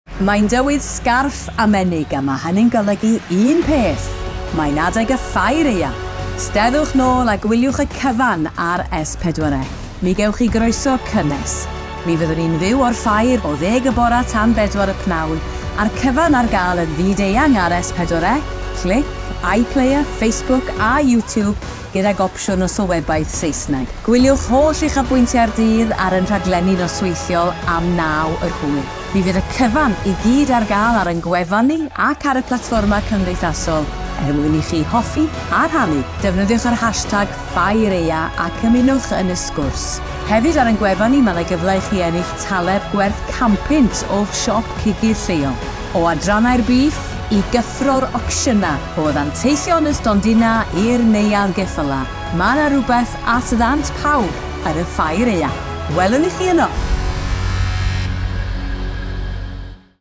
Gwyliwch y Ffair Aeaf YN FYW ar S4C | Watch the Winter Fair LIVE on S4C
Mae’n dywydd sgarff a menyg sy’n meddwl un peth - mae’n adeg y Ffair Aeaf!
It’s scarf and gloves weather, which means one thing - It's time for the Winter Fair!

Gwyliwch y cyfan YN FYW ar S4C | Watch the action LIVE all week on S4C

👉 Y Ffair Aeaf | The Winter Fair
📆 25 - 26 Tachwedd | November